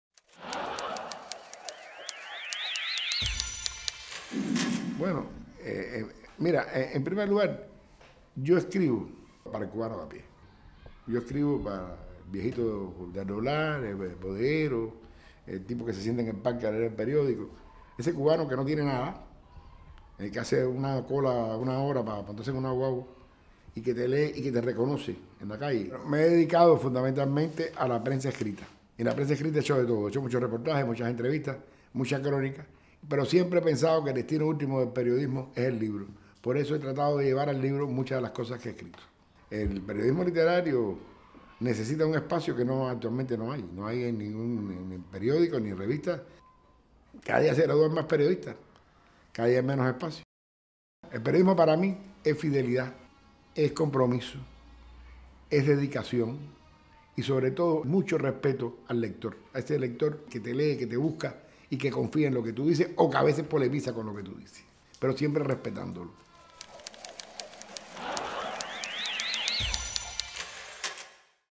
Glorias del Periodismo en Cuba: Ciro Bianchi
Entrevista con Ciro Bianchi, de la serie audiovisual que comenzó a grabarse recientemente para dignificar la labor de los periodistas cubanos, que este año celebran su X Congreso. La producción corre a cargo de jóvenes egresados de la FAMCA y es fruto de la colaboración entre la Unión de Periodistas de Cuba (Upec) y la Asociación Hermanos Saíz (AHS).

Directora: Daniela Muñoz Barroso